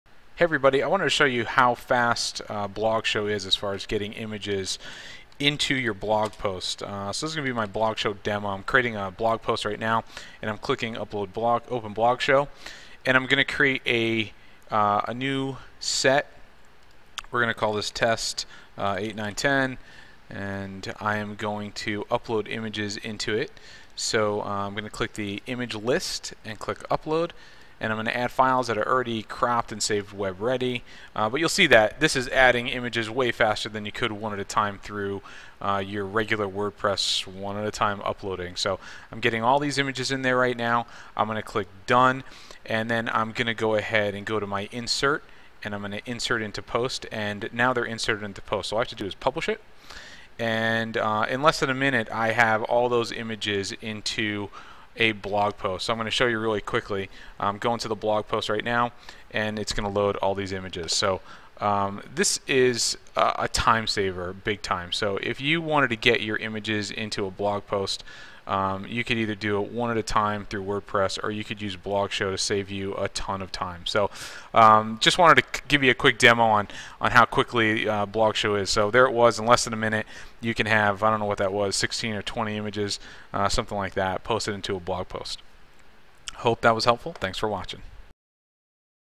Blogshow- saving time bulk uploading
This video shows you how much time you will save posting images into your blog with blogshow. Intothedarkroom.com